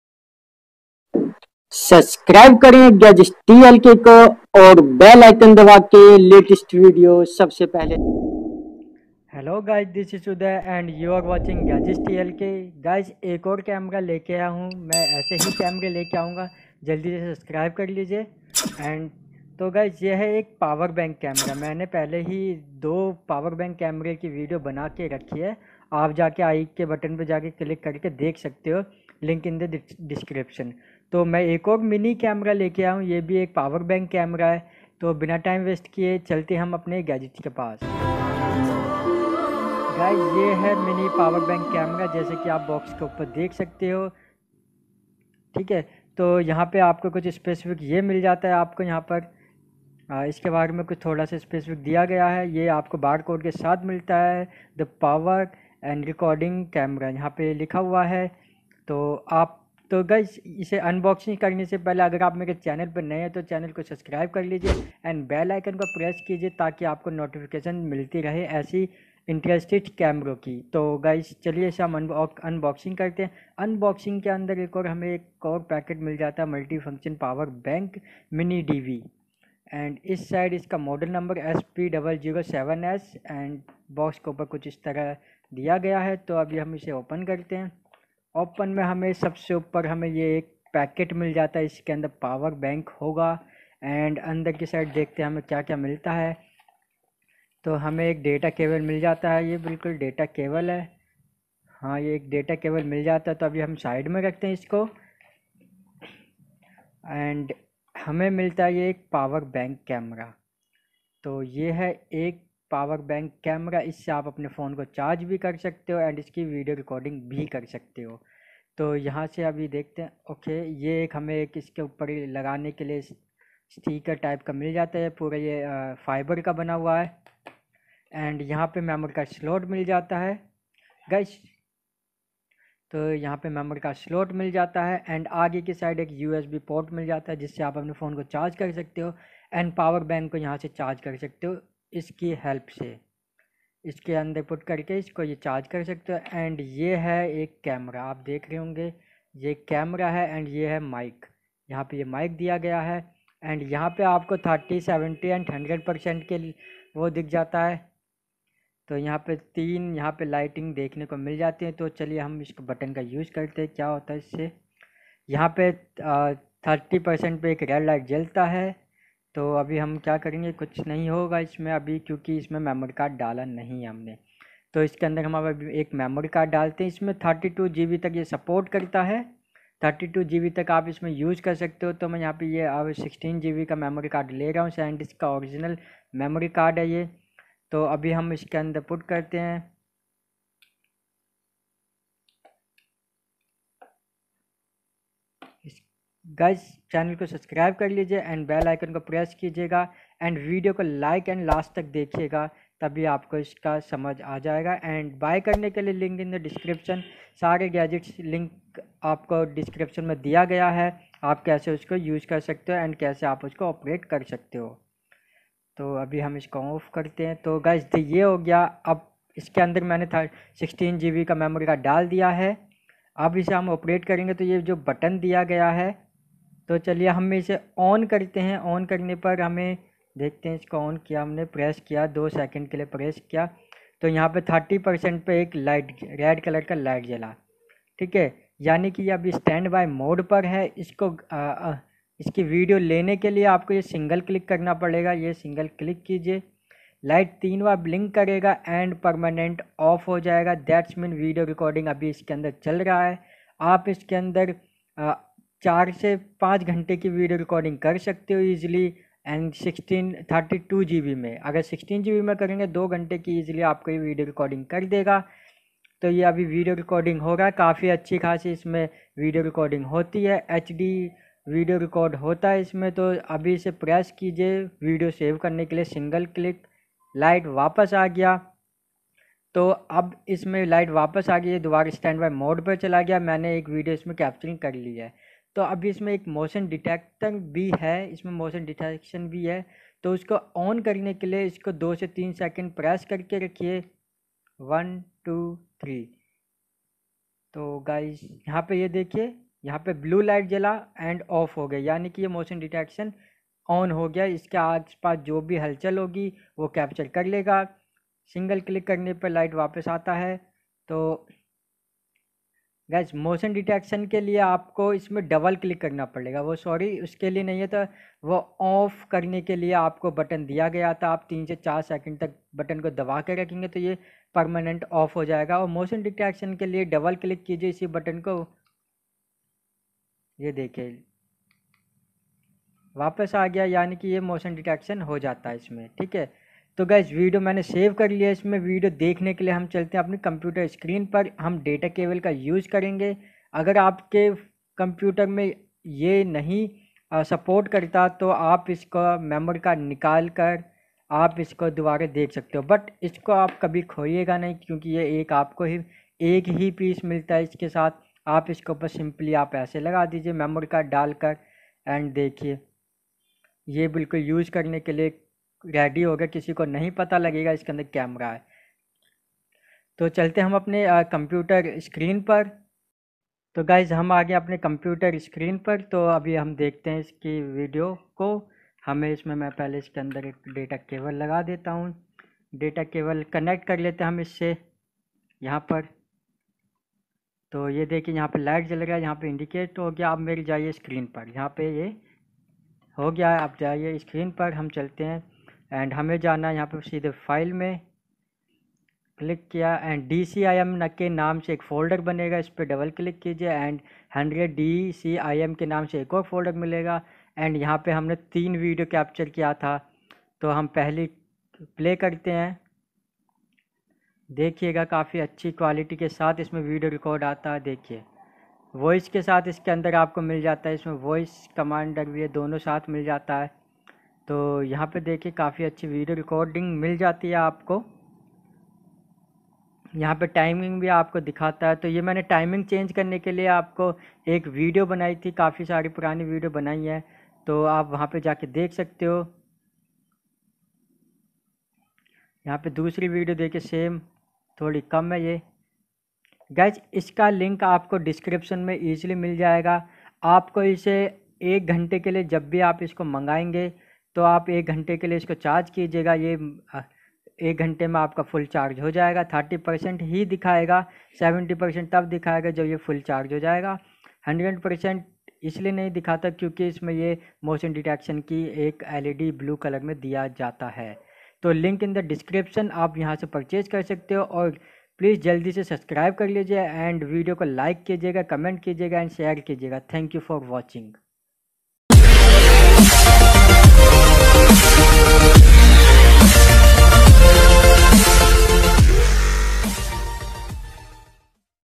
Mini Power Bank Hidden Portable Spy Camera Operation Guide HINDI By Gadgets TLK 🔥
Mini Power Bank Hidden Portable Spy Camera Operation Guide By Gadgets TLK 🔥

Buy Link
this is the best product ever

Product Description
COVERTLY RECORD THE ACTION WHILE ON THE GO Portable & Undetectable Looking for a covert portable hidden camera to secretly record conversations & more while on the go? Then this power bank hidden camera is just what you need! We’ve fitted a high-quality camera inside a fully-functional power bank for charging electronic devices. In an everyday use device such as a power bank, the camera is truly undetectable and no one will suspect a hidden camera when placed on the table during a meeting. No Grainy Videos The camera for spying records video in either 480P, 720P, or 1080p, full-color, and at 30FPS, so you’ll manage to pick out even the tiniest of details. And you’ll enjoy quite a bit of storage space too. With a record rate of 3GB per hour, and support of microSD cards up to 16GB (not included), it will provide a long time of continuous recording. You can pick between continuous and motion-activated recording that prevents idle footage. Here’s why you’ll love this portable spying camera: - Small & Portable, fits in everywhere pocket - Charge your phone & other electronic devices on the go - Quick to set up with no 3rd party software required. - Compatible with Macs & PCs. - HD recording with 30FPS and 1280x720 resolution - Battery Capacity 3000 mAh

My all video check out 1

All Video check out now